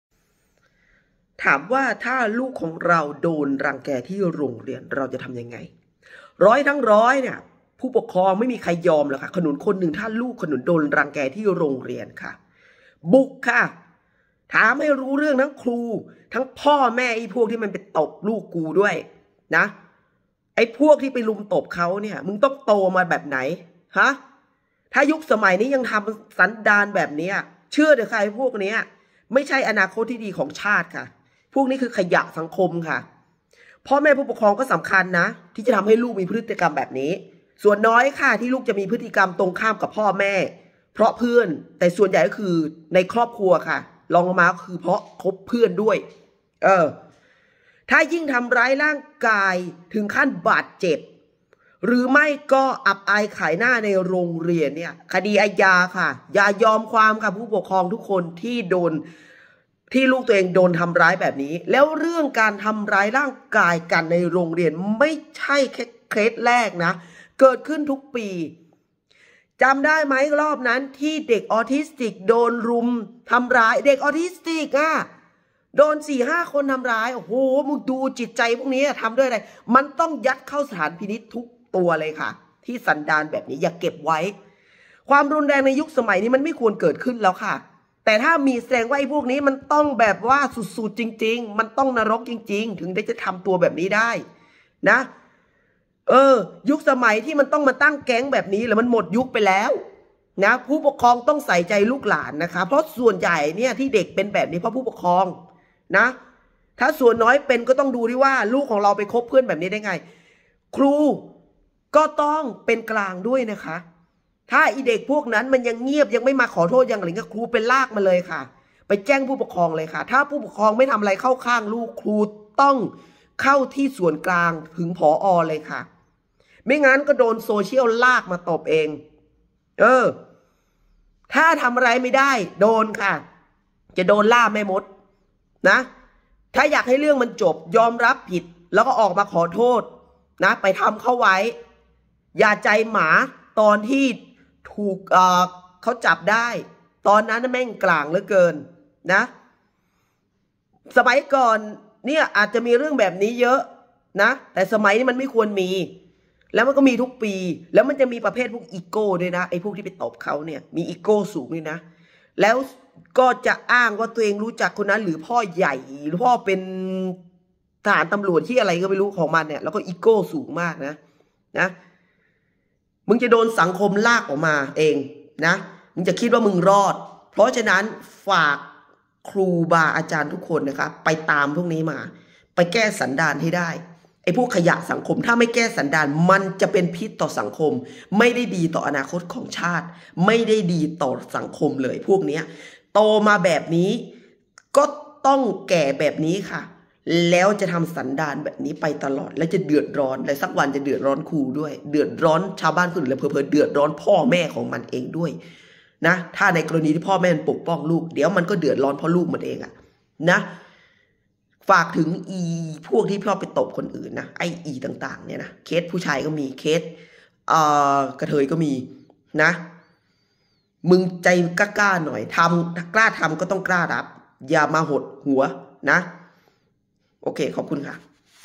กรณี​นักเรียนรุมตบนักเรียน​ในห้องเรียน ขยะสังคมพวกนี้ต้องจัดการให้เด็ดขาด
กรณี​นักเรียนรุมตบนักเรียน​ในห้องเรียน ขยะสังคมพวกนี้ต้องจัดการให้เด็ดขาด กฎหมายเยาวชนต้อง หนักแน่นกว่านี้ พฤติกรรมแบบนี้ไม่ใช่อนาคตที่ดีของชาติ มันคือภาระ​สังคม​ในอนาคต​ ถ้าไม่ดัดสันดานอย่างจริงจังตอนนี้ โตไปก็จะสร้างแต่ปัญหา